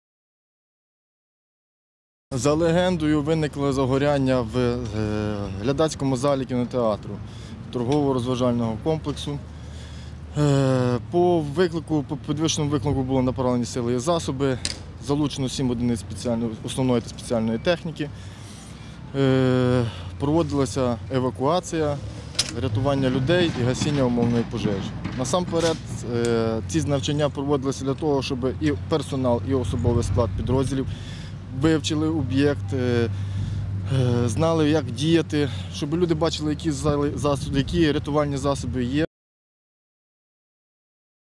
Умовну пожежу сьогодні ліквідували у ТРЦ «Вудмолл»
Близько півсотні працівників та відвідувачів  сьогодні евакуювали із торгово-розважального центру « Вудмолл» у Хмельницькому. Так рятувальники обласного відомства спільно з персоналом закладу вчилися, як діяти під час пожежі. Нагадаю: на Хмельниччині тривають позапланові перевірки закладів масового перебування людей. Працівники обласного управління ДСНС обстежують стан автоматичної системи протипожежного захисту.